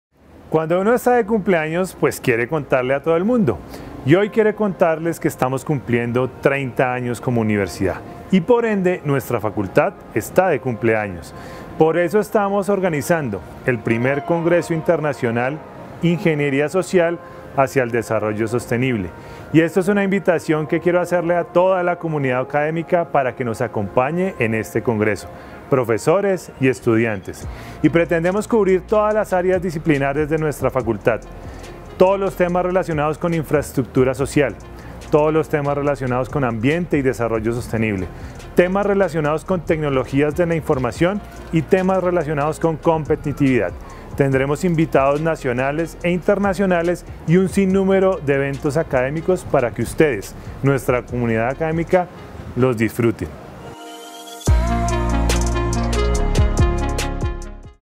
Participa: I Congreso Internacional de Ingeniería Social: Una apuesta hacia el desarrollo sostenible
Desde la Facultad de Ingeniería de UNIMINUTO Bogotá - Presencial, queremos hacer extensiva la invitación a participar en el I Congreso Internacional de Ingeniería Social: Una apuesta hacia el desarrollo sostenible.

Te esperamos sin falta del 31 de agosto al 2 de septiembre de 2022.

Conoce más información de nuestro I Congreso Internacional de Ingeniería, ingresando al siguiente sitio web